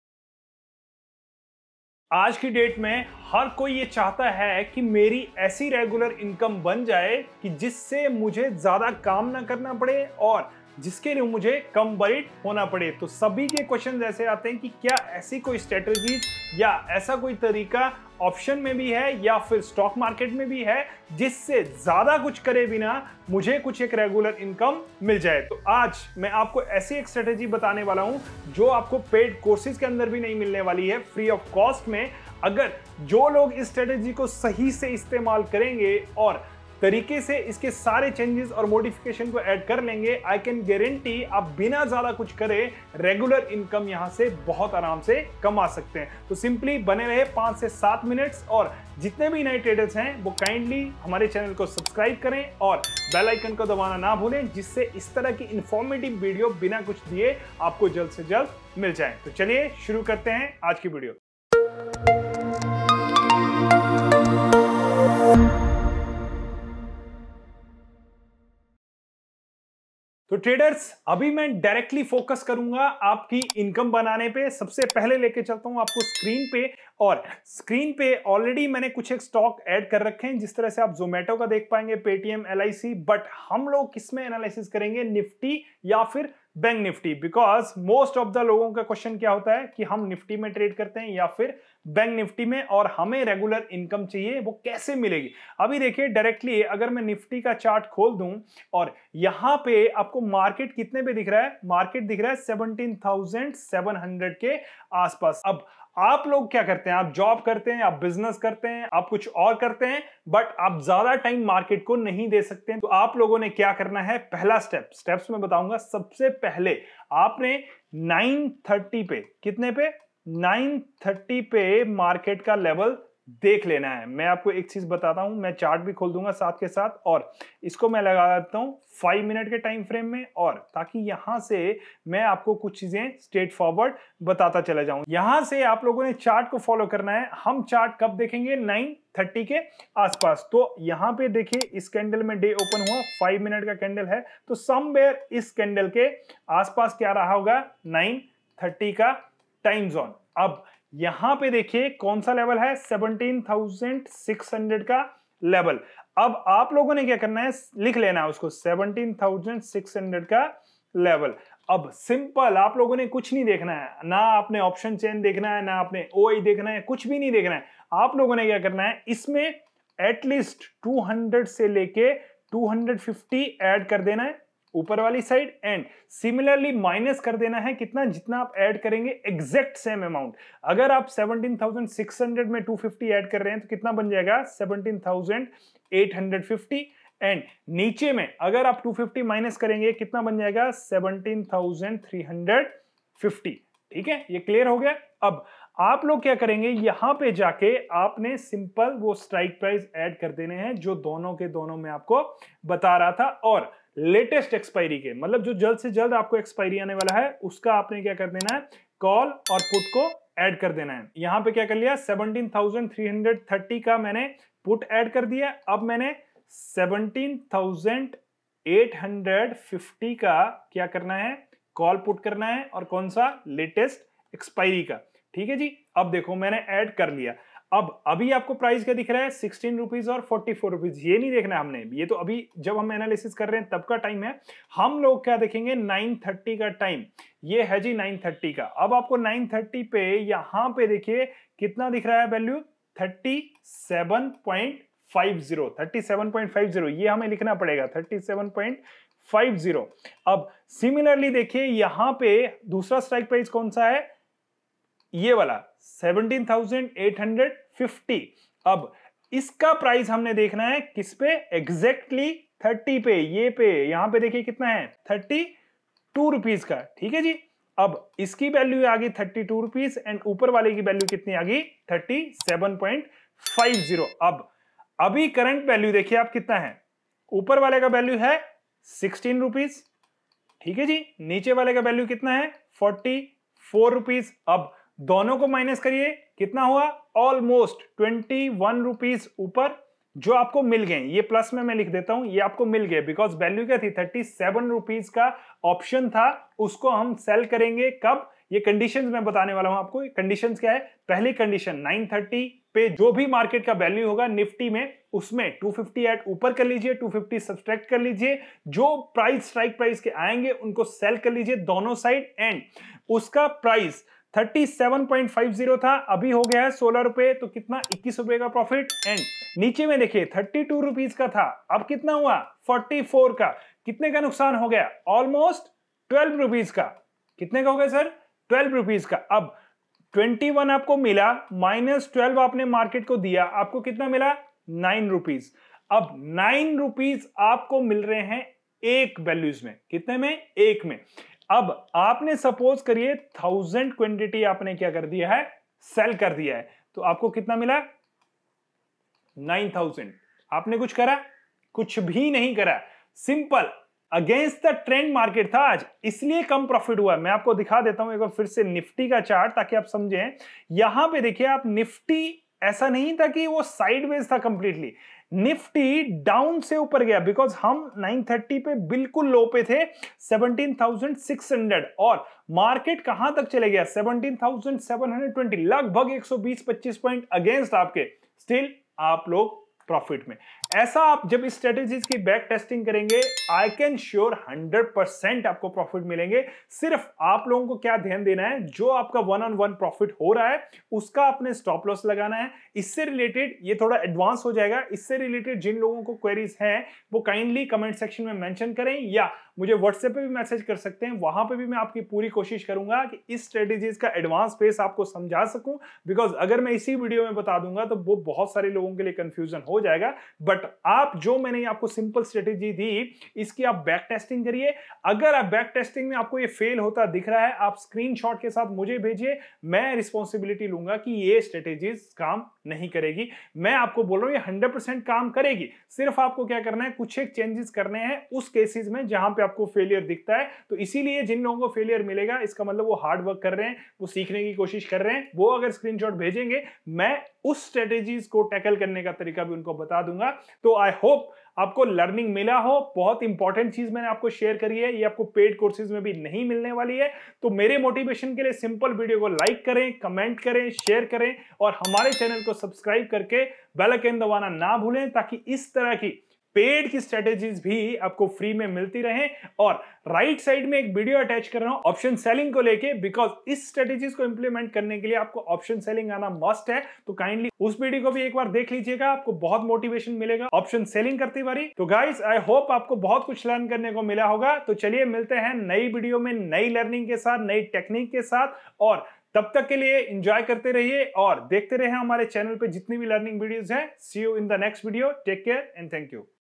Option Selling vs Option Buying | Option Trading Strategies | Intraday Trading Strategies in Hindi
Who earns more? Option Buyers Or Option Sellers? Regular Income by Option Strategy